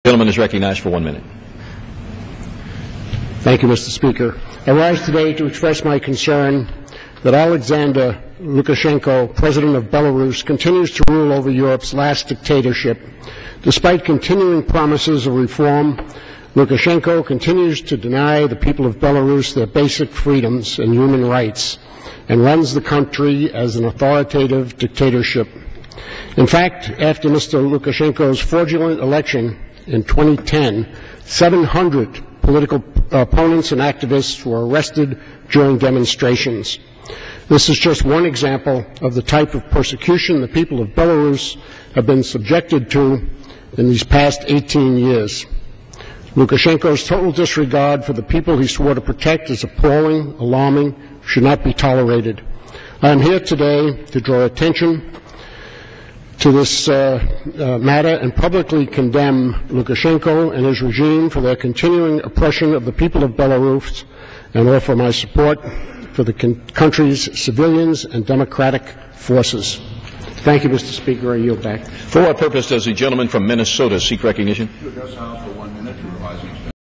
Turner Speaks Out Against Belarusian Dictatorship
Congressman Bob Turner (NY-09) spoke out against Belarusian dictator Alexander Lukashenko and the countless human rights violations carried out by his regime.